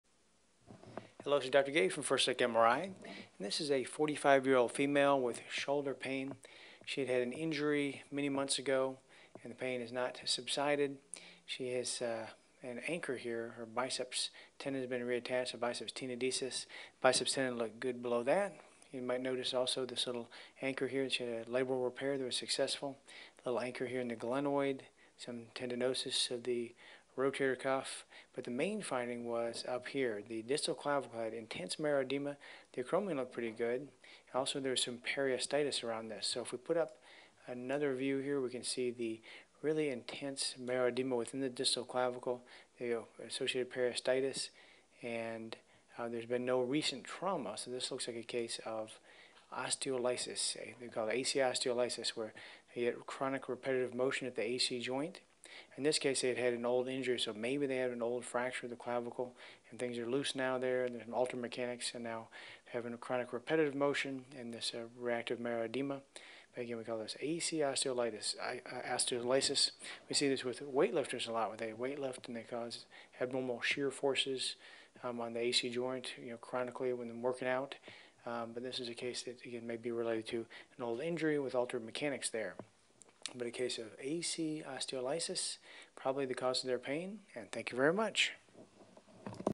AC Osteolysis
Marrow edema and bone resorption about the acromioclavicular joint is termed AC osteolysis because of the lucency that was present about the AC joint on plain x-rays.  The reactive bony changes are related to repetitive motion and/or altered mechanics of stress at the AC joint from weight lifting or a repetitive task.